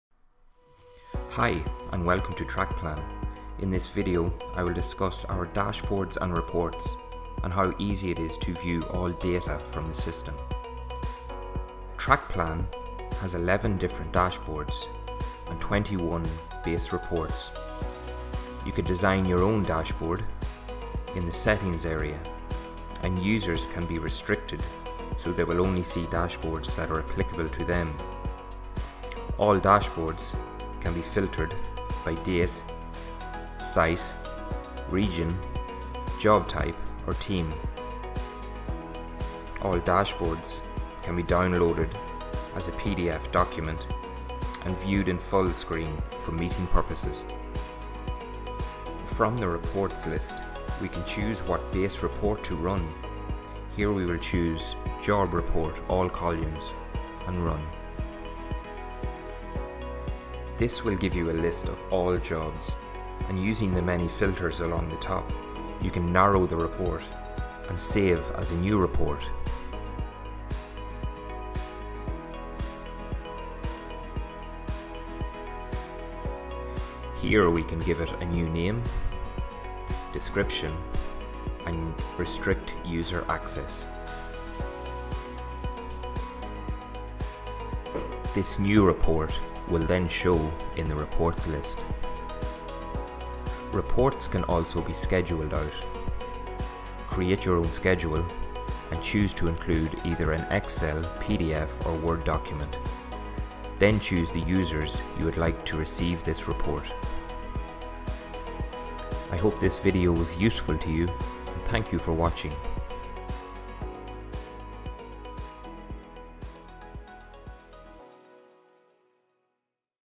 Dashboard & Reports - Facilities Management Software
This video shows you how to navigate the Dashboard and get a great overview of maintenance and compliance across your estate. Trackplan has 11 dashboards and 22 base reports to give users and and stakeholders their own relevant analysis and view.